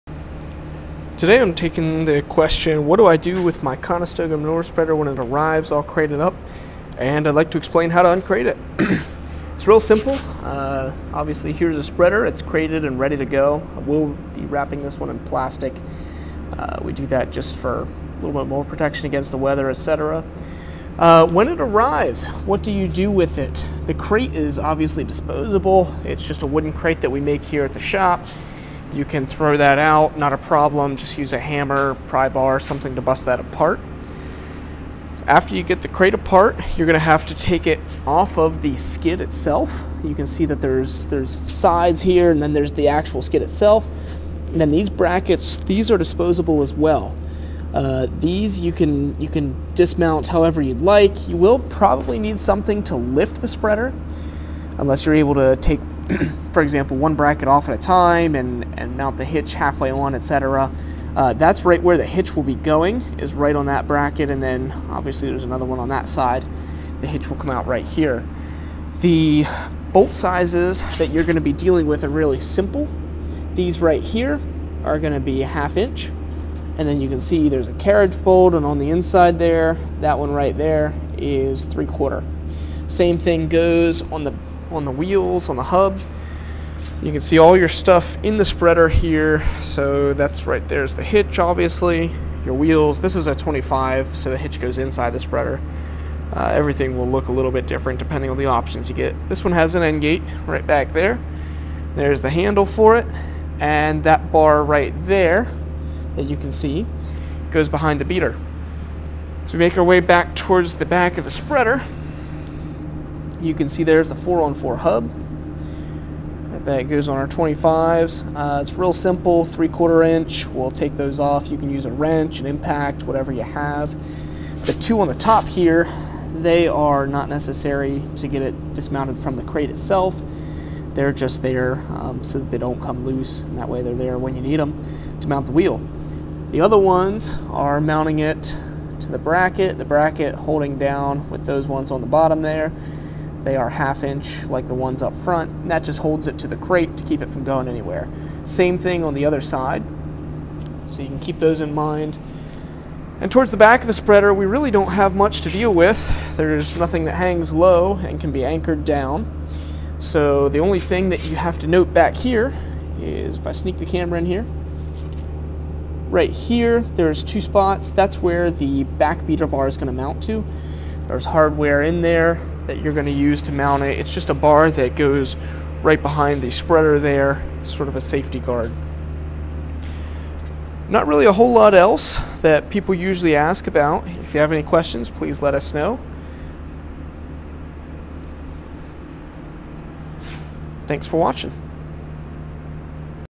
How to Uncrate a Conestoga Manure Spreader (CM-25)
Here’s a simple explanation of how to uncrate a spreader.
Tools necessary listed below:

-Wrench, socket, or impact for 1/2” & 3/4”
-Hammer, pry bar, etc. to open crate (it’s yours to do as you wish)
-Forklift, front loader, jack, pry bar to lift the spreader up for hitch/wheels & tires.